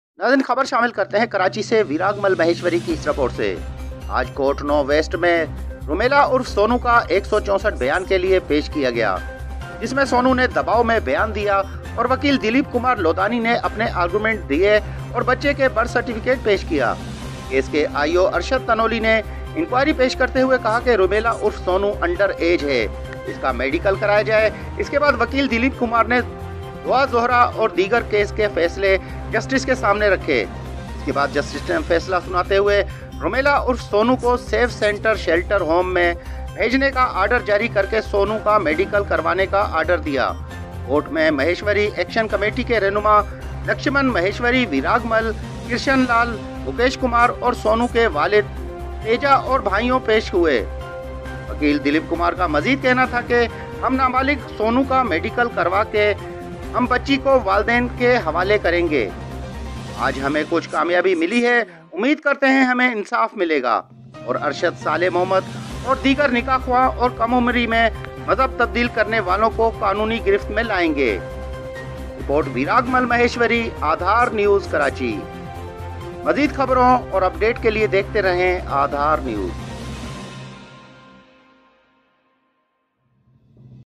آج کورٹ 9 ویسٹ مین رمیلا عرف سونو کا 164 بیان کے لیے پیش کیا گیا جس مین سونو نے دباٶ مین بیان
رپورٹ ویراگ مل مھیشوری

آج کورٹ 9 ویسٹ مین رمیلا عرف سونو کا 164 بیان کے لیے پیش کیا گیا جس مین سونو نے دباٶ مین بیان دیا اور وکیل دلیپ کمار لودانی نے اپنے آرگیومیٹ دیے اور بچے کے برتھ سرٹیفکیٹ پیش کیا کیس کے آٸی او ارشد تنولی نے انکواٸیری پیش کرتے ہوۓ کہا رمیلا عرف سونو انڈر ایج ہے اس کا میڈیکل کرایا جاۓ اس کے بعد وکیل دلیپ کمار نے دعا زھرہ اور دیگر کیس کے فیصلے جسٹس کے سامنے رکھے  اس کے بعد جسٹس نے فیصلا سناتے ہوۓ رمیلا عرف سونو کو سیو سینٹر ۔شیلٹر ھوم مین بیھجنے کا آرڈر جاری کرکے سونو کا میڈیکل کروانے کا آرڈر دیا کورٹ مین مھیشوری ایکشن کمیٹی کے رھنما ٕ لکشمن مھیشوری۔ ویراگ مل۔ کرشن لال۔ ۔ مکیش کمار اور سونو کے والد تیجا اور بھاٸیون پیش ہوۓ وکیل دلیپ کمار کا مزید کہنا تھا کہ ہم نابالغ سونو کا میڈیکل کرواکے ہم بچی کو واثون کے حوالے کرین گے آج ہمین کچھ کامیابی ملی ہے امید کرتے ہین ہمین انصاف ملے گا اور ارشد صالح محمد اور دیگر نکاح خوان اور کم عمری مین مذھب تبدیل کرنے والون  کو قانونی گرفت مین لاٸین گے